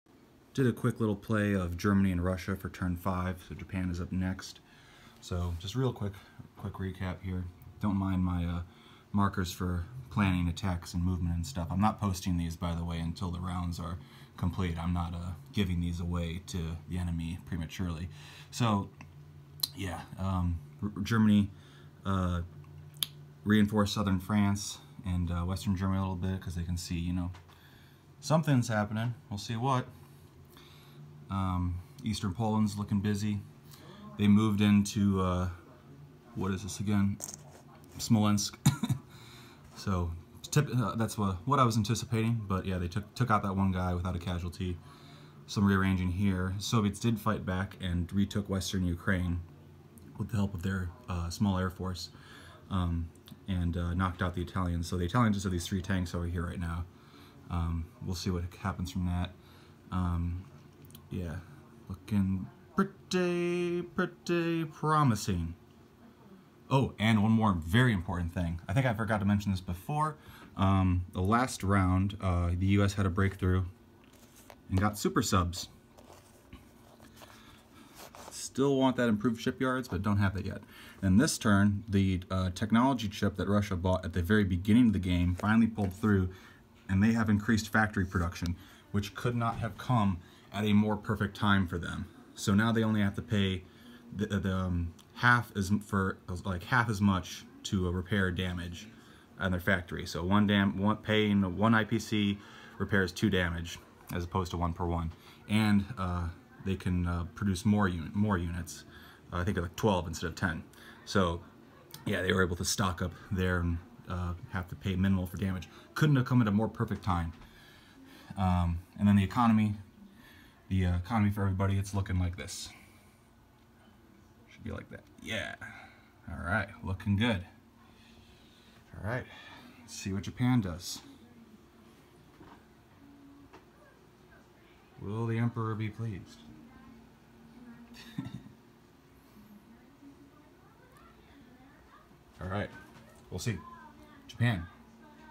[12] OOB G40 Lockdown Edition Round 5 Recap Part 1
Quick recap of Germany and Russia's turns in Round 5.